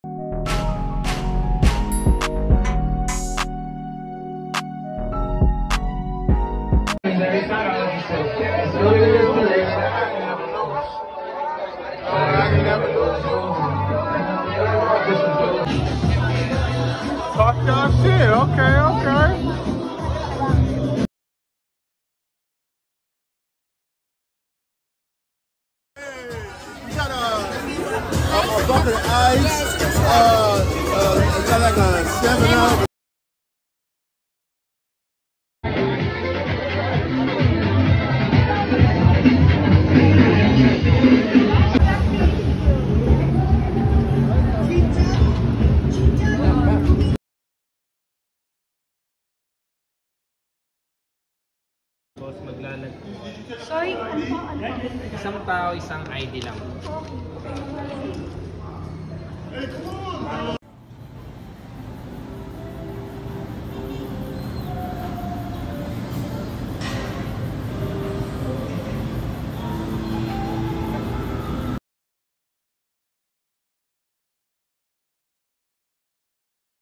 Exploring Manila's Vibrant LA Cafe Scene on My First Day!
LA Cafe in Manila first day exploring the scenery 🇵🇭